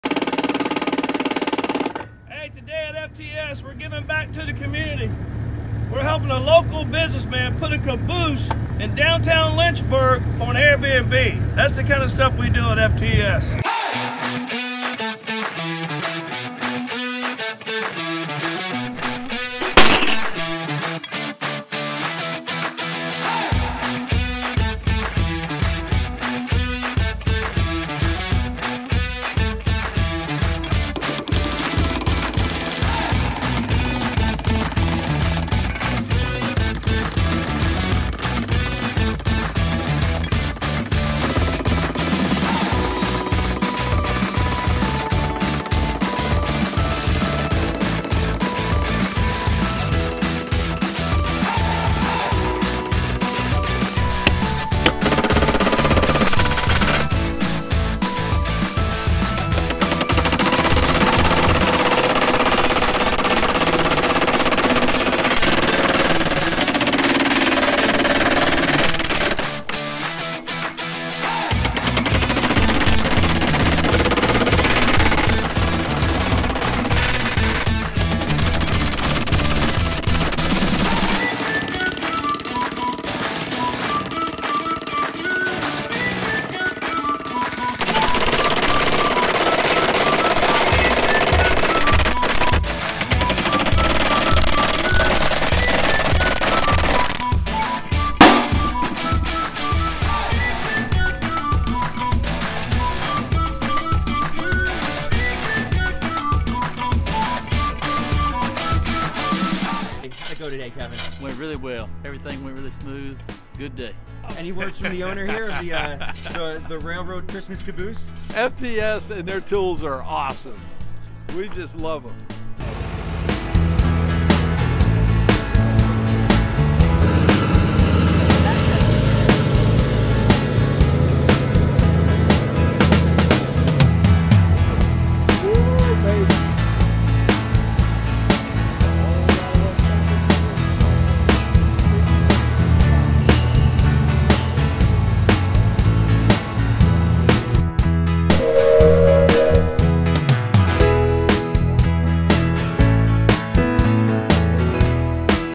𝘾𝙖𝙗𝙤𝙤𝙨𝙚 𝙞𝙣𝙩𝙤 𝘼𝙞𝙧𝙗𝙣𝙗: FTS Giving Back
See how FTS was able to use its battery powered railroad tools to turn a local businessman’s caboose into an Airbnb!